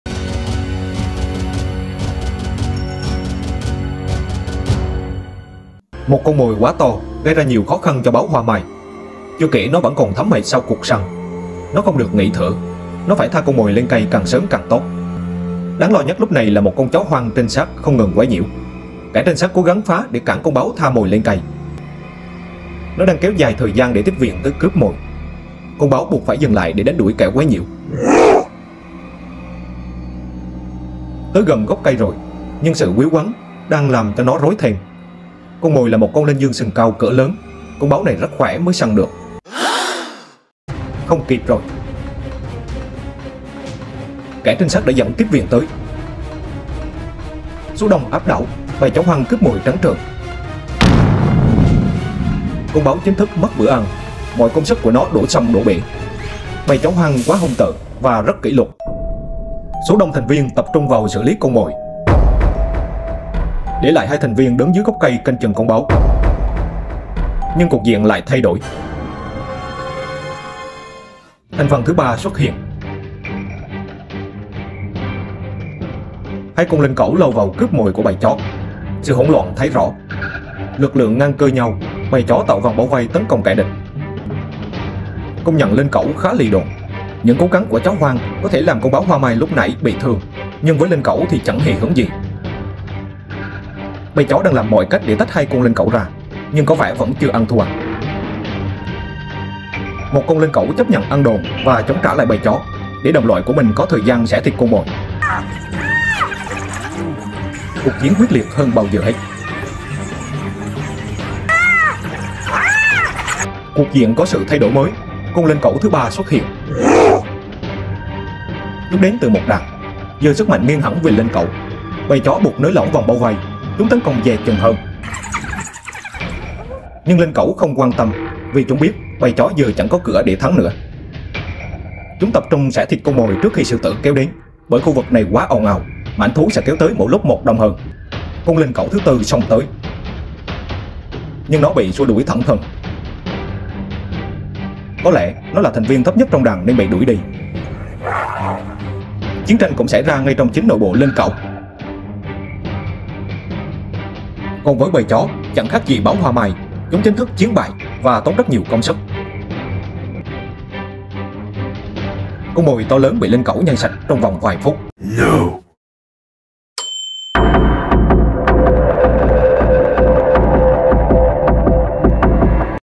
Chuyện gì xảy ra khi BÁO, LINH CẨU, CHÓ HOANG cùng hỗn chiến ?
Một con mồi là linh dương sừng cao, do con báo hoa mai bắt được. Nhưng chó hoang và linh cẩu đã tới để giật lấy. Chuyện hy hữu khi 3 loài hoang dã này đụng độ nhau tại 1 khu vực...